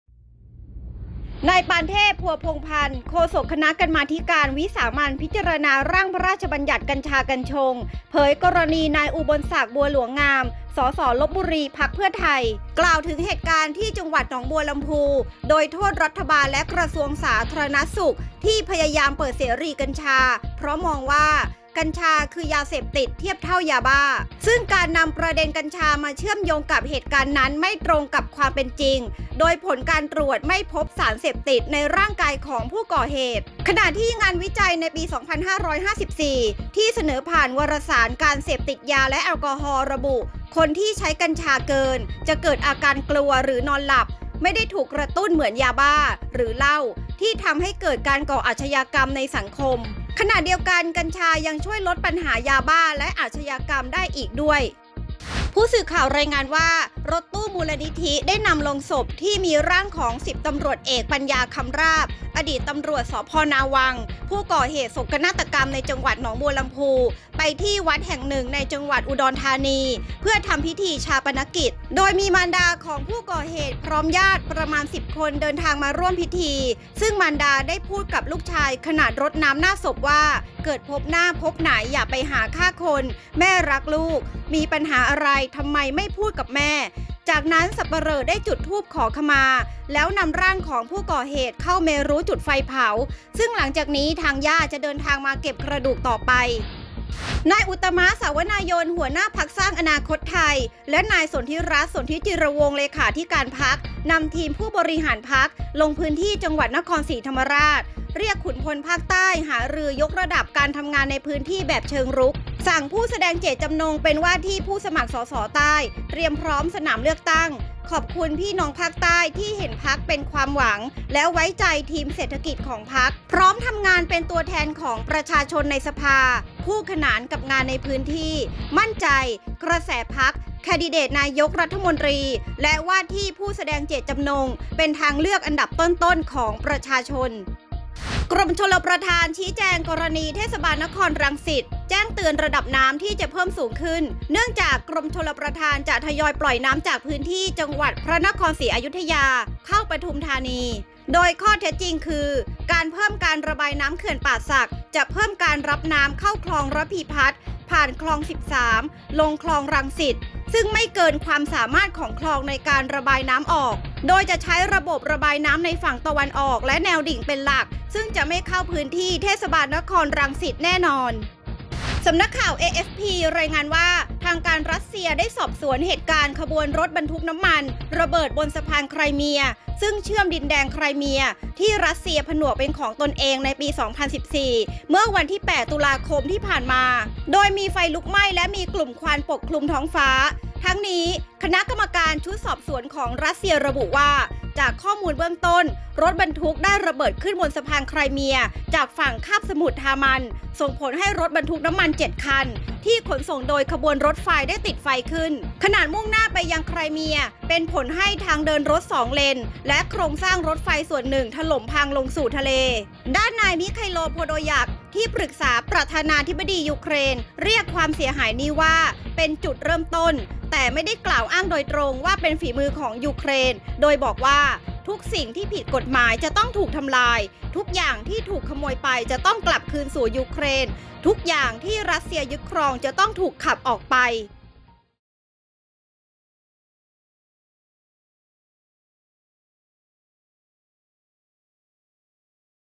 "กัญชา" ไม่ใช่ต้นเหตุ : [NEWS UPDATE]
นายปานเทพ พัวพงษ์พันธ์ โฆษกคณะกรรมาธิการวิสามัญพิจารณาร่างพระราชบัญญัติ กัญชา กัญชง
เผยกรณี นายอุบลศักดิ์ บัวหลวงงาม ส.ส.ลพบุรี พรรคเพื่อไทย กล่าวถึงเหตุการณ์ที่จังหวัดหนองบัวลำภู โดยโทษรัฐบาลและกระทรวงสาธารณสุข ที่พยายามเปิดเสรีกัญชา เพราะมองว่ากัญชาคือยาเสพติดเทียบเท่ายาบ้า
ซึ่งการนำประเด็นกัญชามาโยงกับเหตุการณ์นั้นไม่ตรงกับความเป็นจริง โดยผลการตรวจไม่พบสารเสพติดในร่างกายของผู้ก่อเหตุ
ขณะที่งานวิจัยในปี 2554 ที่เสนอผ่านวารสารการเสพติดยาและแอลกอฮอล์ ระบุ คนที่ใช้กัญชาเกิน จะเกิดอาการกลัวหรือนอนหลับ ไม่ได้ถูกกระตุ้นเหมือนยาบ้าหรือเหล้าที่ทำให้เกิดการก่ออาชญากรรมในสังคม ขณะเดียวกัน กัญชายังช่วยลดปัญหายาบ้าและอาชญากรรมได้อีกด้วย

2.เผาร่างอดีต ส.ต.อ. คลั่ง

3.ติวขุนพลใต้ทำงานเชิงรุก

4.ยืนยันน้ำไม่เข้ารังสิต

5.รัสเซียเร่งสอบเหตุระเบิด

id=com.sondhitalk.asia.android&fbclid=IwAR0_8wTAnDmJz31oRwo7tA6q9rzVpQjuwAFhg0uzmTG-4c2c2KBFKwt-HK4